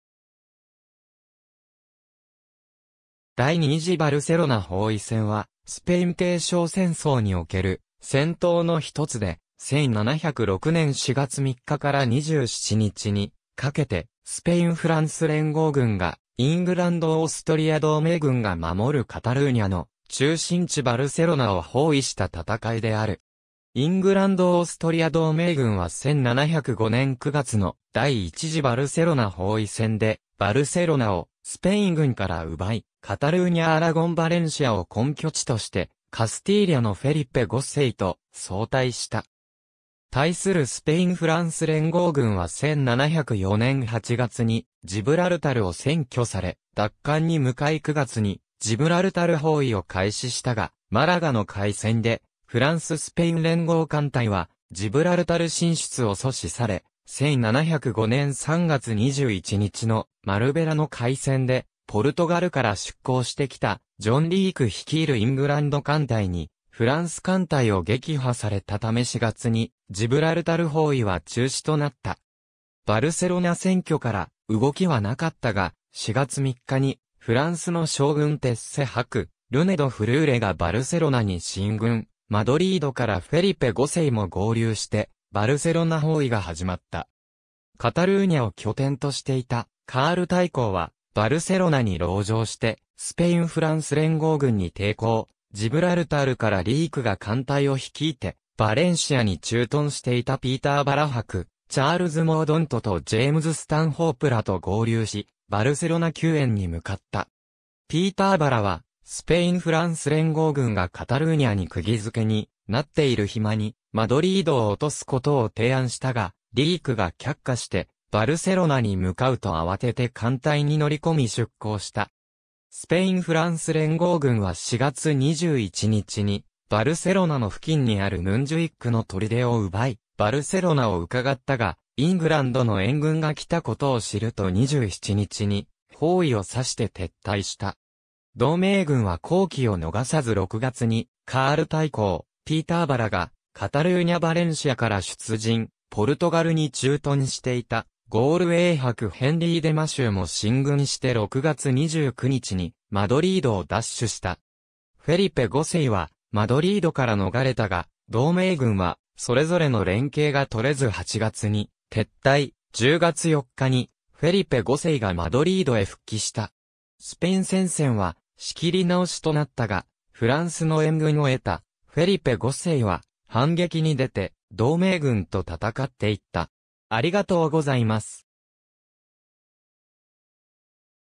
バルセロナ包囲戦 (1706年)
第2次バルセロナ包囲戦（カタルーニャ語：Setge de Barcelona）は、スペイン継承戦争における戦闘の一つで、1706年4月3日から27日にかけてスペイン・フランス連合軍がイングランド・オーストリア同盟軍が守るカタルーニャの中心地バルセロナを包囲した戦いである。
イングランド・オーストリア同盟軍は1705年9月の第1次バルセロナ包囲戦でバルセロナをスペイン軍から奪い、カタルーニャ・アラゴン・バレンシアを根拠地としてカスティーリャのフェリペ5世と相対した。
対するスペイン・フランス連合軍は1704年8月にジブラルタルを占拠され（ジブラルタルの占領）、奪還に向かい9月にジブラルタル包囲を開始したが、マラガの海戦でフランス・スペイン連合艦隊はジブラルタル進出を阻止され、1705年3月21日のマルベラの海戦でポルトガルから出航してきたジョン・リーク率いるイングランド艦隊にフランス艦隊を撃破されたため4月にジブラルタル包囲は中止となった。
バルセロナ占拠から動きは無かったが、4月3日にフランスの将軍テッセ伯ルネ・ド・フルーレがバルセロナに進軍、マドリードからフェリペ5世も合流してバルセロナ包囲が始まった。
カタルーニャを拠点としていたカール大公はバルセロナに籠城してスペイン・フランス連合軍に抵抗、ジブラルタルからリークが艦隊を率いてバレンシアに駐屯していたピーターバラ伯チャールズ・モードントとジェームズ・スタンホープらと合流しバルセロナ救援に向かった。
ピーターバラはスペイン・フランス連合軍がカタルーニャに釘付けになっている隙にマドリードを落とすことを提案したが、リークが却下してバルセロナに向かうと慌てて艦隊に乗り込み出航した。
スペイン・フランス連合軍は4月21日にバルセロナの付近にあるムンジュイックの砦を奪いバルセロナを窺ったが、イングランドの援軍が来たことを知ると27日に包囲を注して撤退した。
同盟軍は好機を逃さず6月にカール大公・ピーターバラがカタルーニャ・バレンシアから出陣、ポルトガルに駐屯していたゴールウェイ伯ヘンリー・デ・マシューも進軍して6月29日にマドリードを奪取した。
フェリペ5世はマドリードから逃れたが、同盟軍はそれぞれの連携が取れず8月に撤退、10月4日にフェリペ5世がマドリードへ復帰した。
スペイン戦線は仕切り直しとなったが、フランスの援軍を得たフェリペ5世は反撃に出て同盟軍と戦っていった。